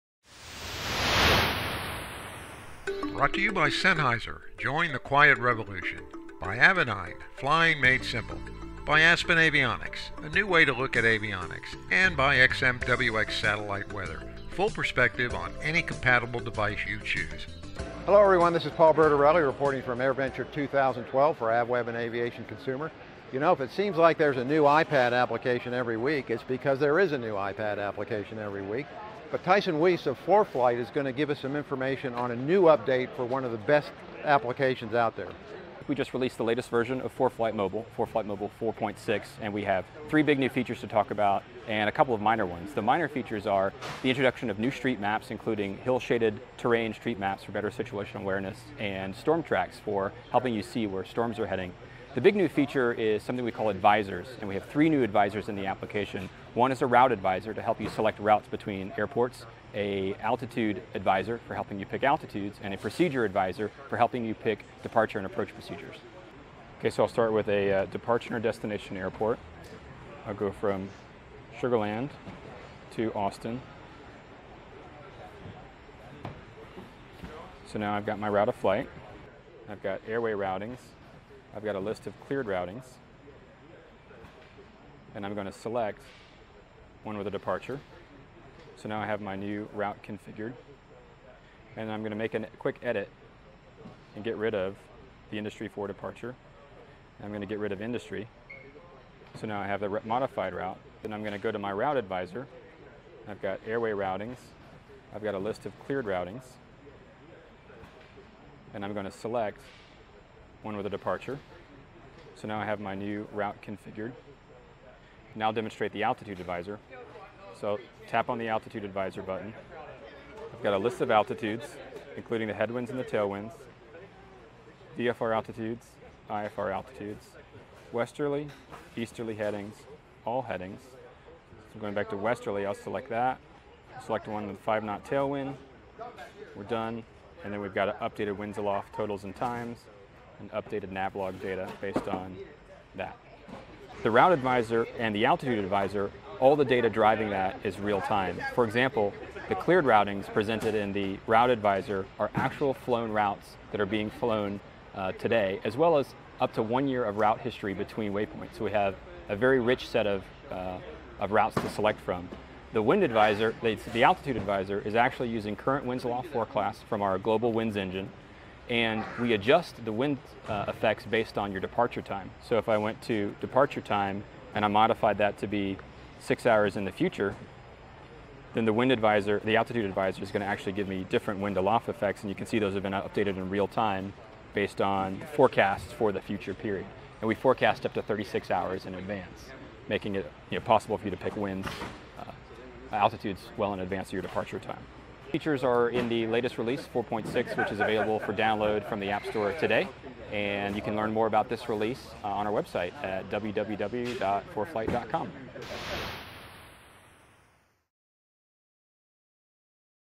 Foreflight's New Advisor Update
As the leading aviation flight planning app, Foreflight has been a mile a minute with revisions and improvements. Here at AirVenture 2012, they've announced a new set of revisions that include sophisticated but easy-to-use routing and altitude functions.